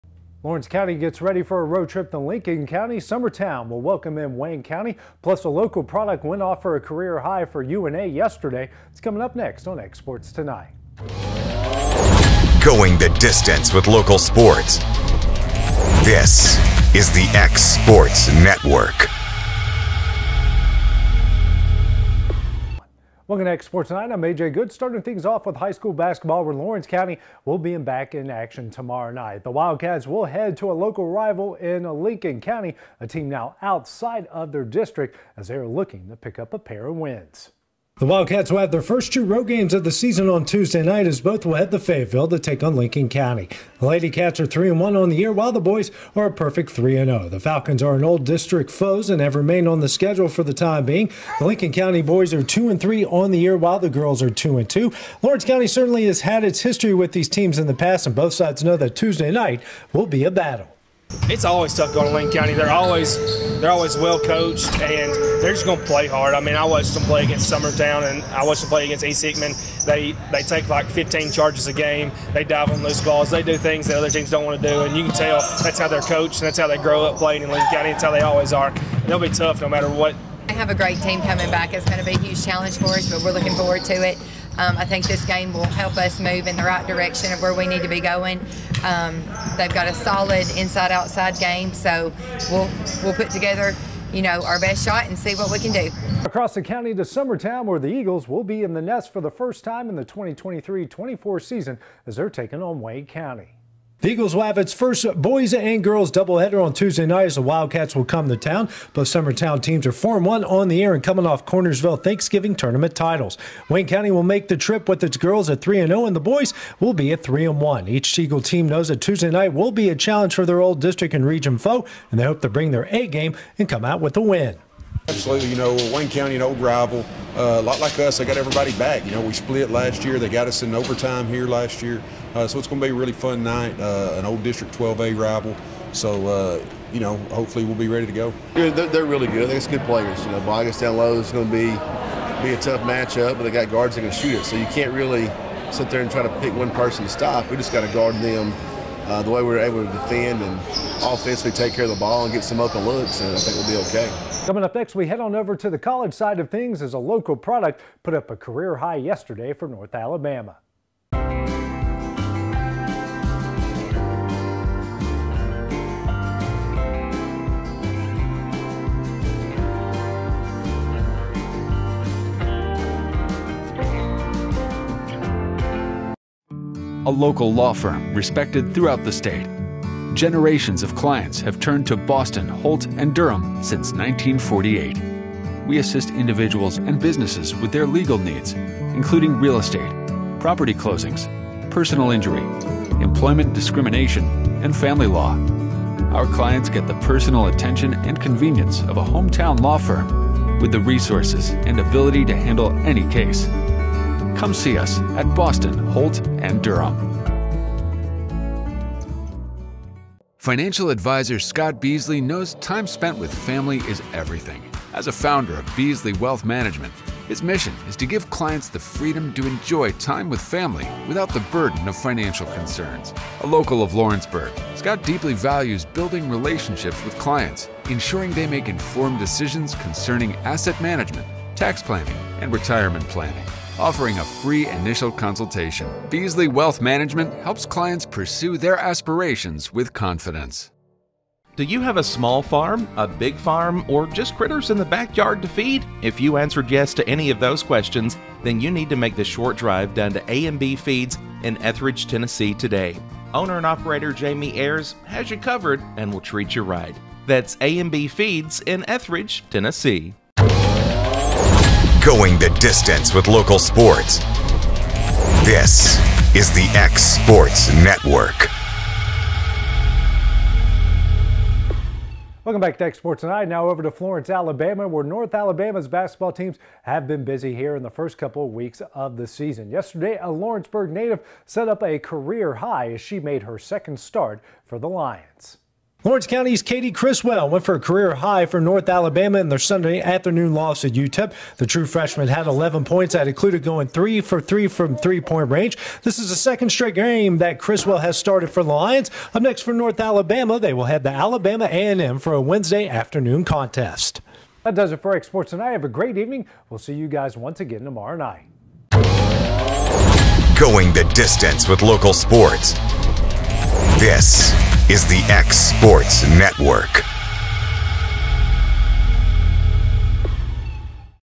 X Sports Tonight 11-27
Coming up on X Sports Tonight.

Lawrence Co Basketball prepares for a road trip to Lincoln Co
Summertown gets ready to host Wayne Co
Plus, Katie Criswell has a career high for UNA on Sunday